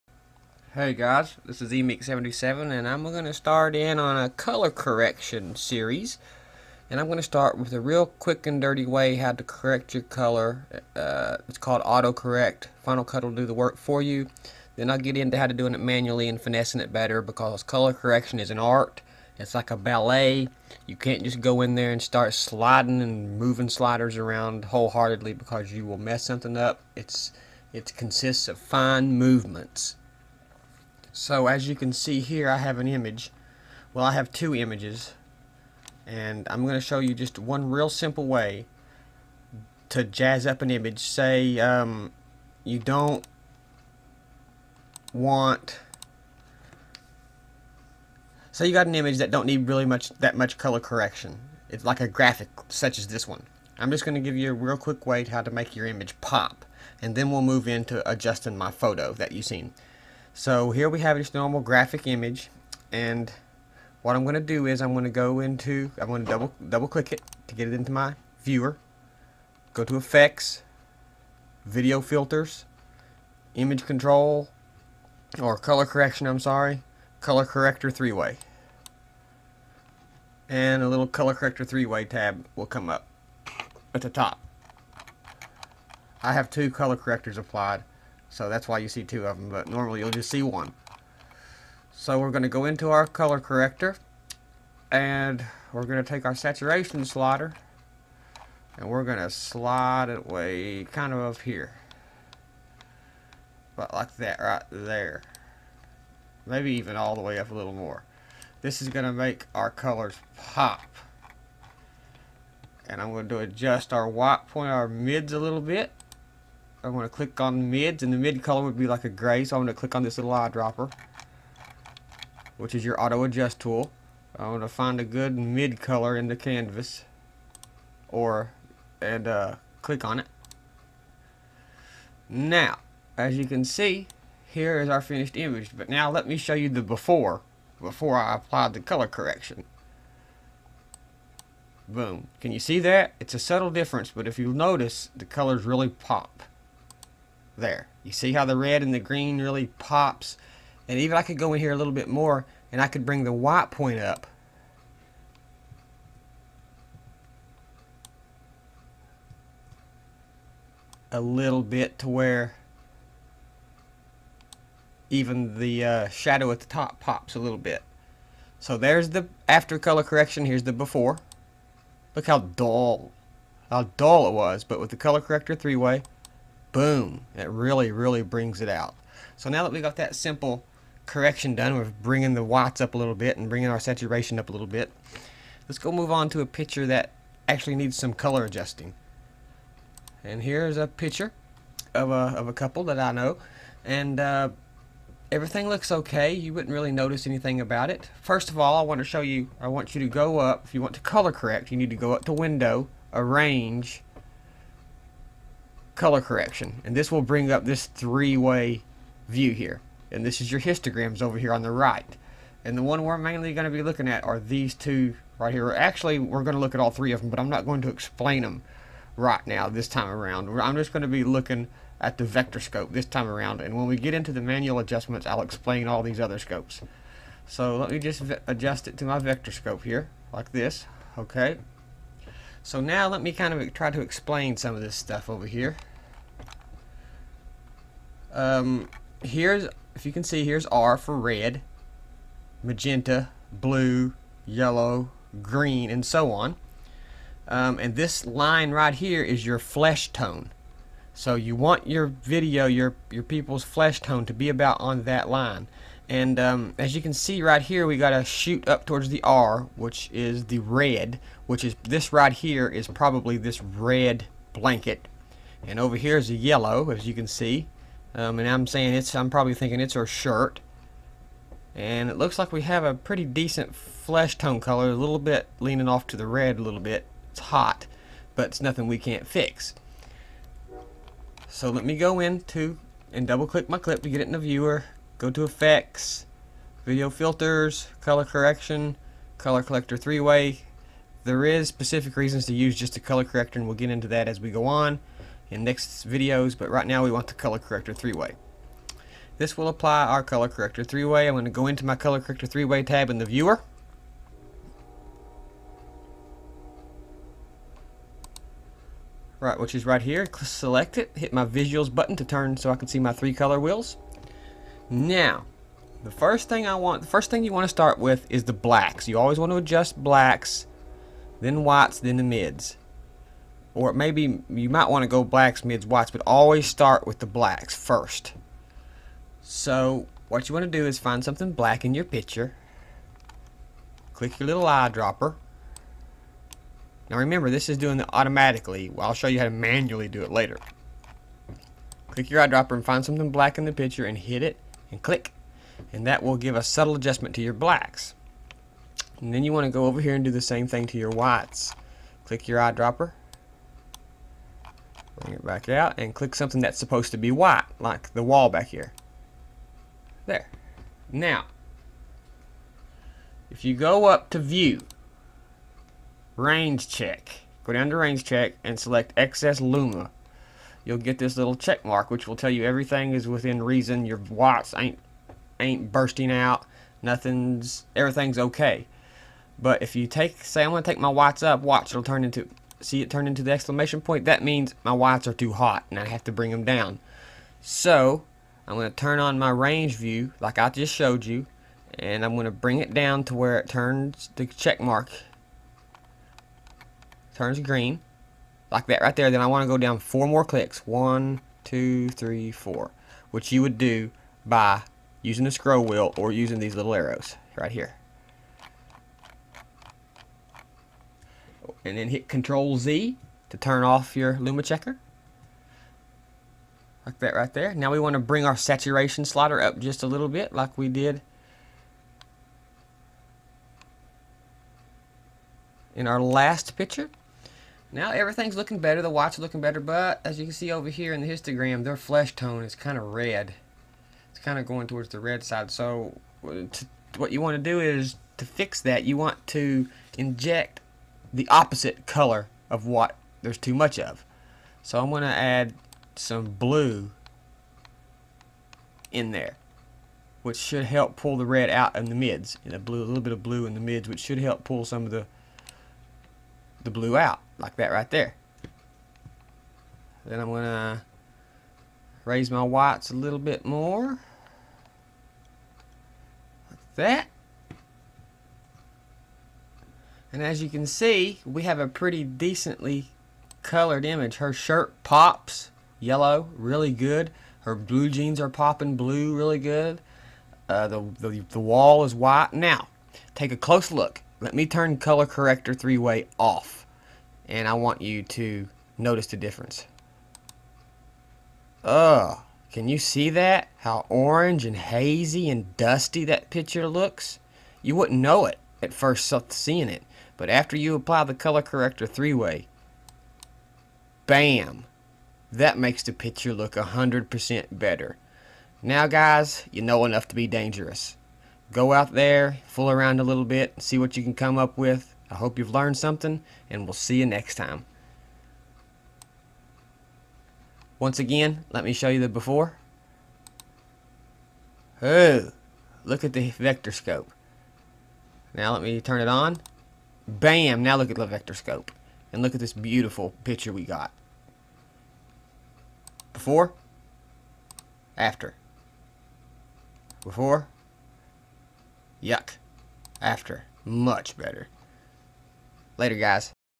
FInal Cut Pro Tutorial: Using The Color Corrector 3 way
How to use the color corrector 3 way in FCP. Now that Im a partner i can post more of my older tutorials that are over 10 minutes or to big or both.